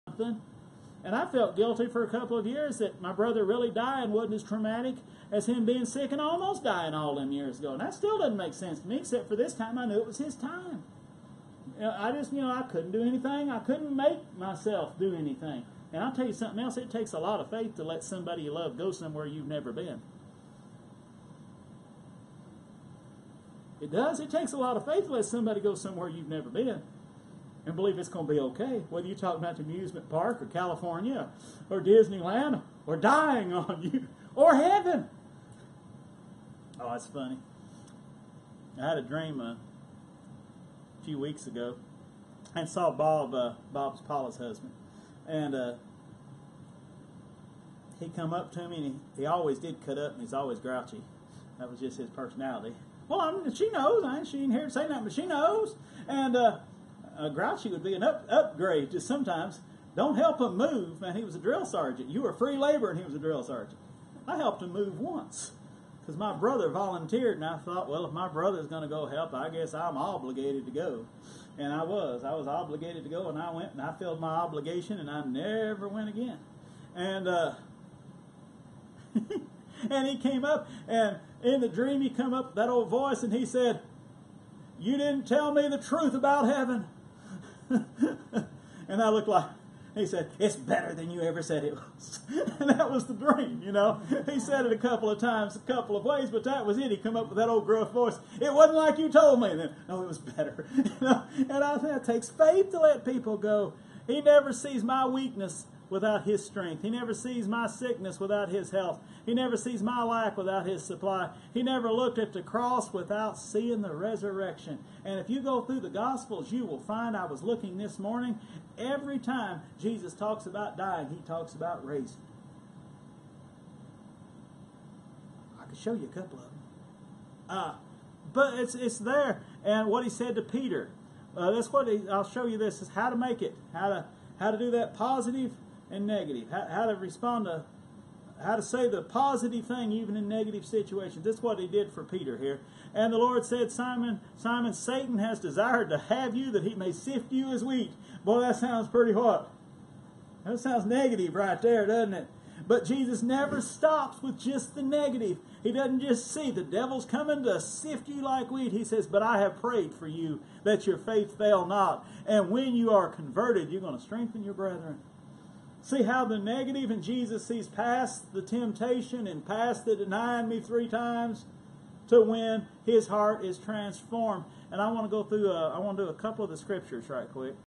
It Takes A Lot of Faith to Let Someone Go !
Have you had to go through or are walking Through the experience of "Letting a Loved One Go" ?
It "takes a Lot of Faith " to "Let Someone Go ", Where you have Never Been Yourself . I have walked there ... one time my brother was miraculously healed of cancer then years latter (22 years) I had to watch him make the journey from earth to heaven ... I could not "force him to stay here any longer ... it Takes a Lot of Faith to Let them go ... Ahead where we have Never been !!!  God Loves you and wants to heal your heart
Pastor Len